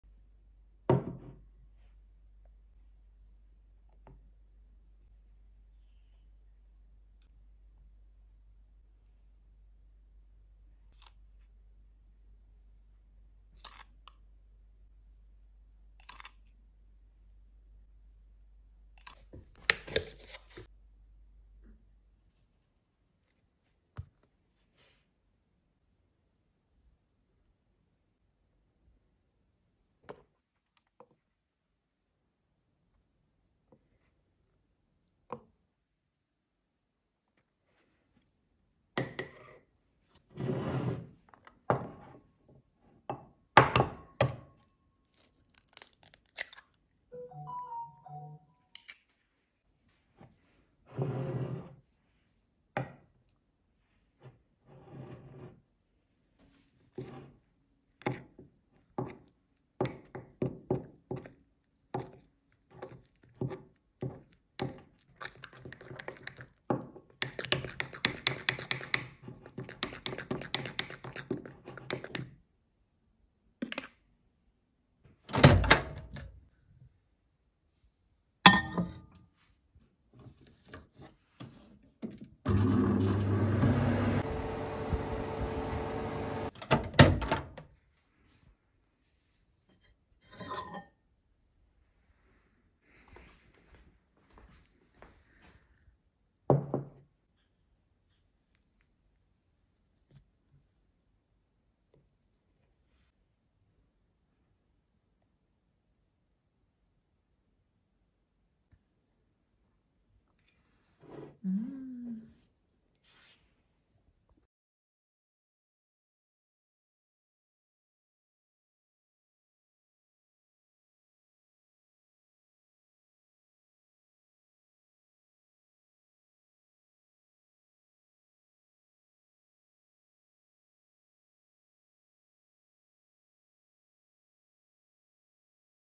Le MUG Cake au Nesquik😍Prêt en 2 minutes(facile et rapide)
Le MUG Cake au Nesquik

4 cas de farine/ tasse
4 tblspn of flour/ mug
4 cas de sucre/ tasse
4 tblspn of sugar/ mug
1 cas d’huile/ tasse
1 tblspn oil/ mug
2 cas de nesquik/ tasse
2 tblspn of nesquik/ mug
2 cas de lait/ tasse
2 tblspn of milk/ mug
1 œuf/ tasse
1 egg/ mug

2 min environ / micro onde
2 min or so/ microwave